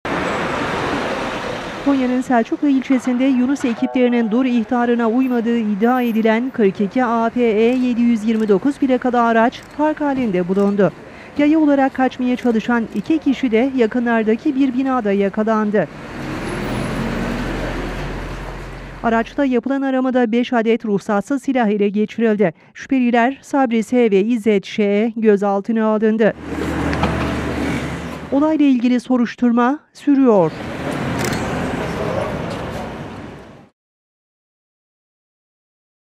2 şüpheli gözaltında!
Konya’da şüphe üzerine durdurulan araçta yapılan aramada 5 adet ruhsatsız silah ele geçirildi. Sürücü ve yanındaki arkadaşı gözaltına alındı.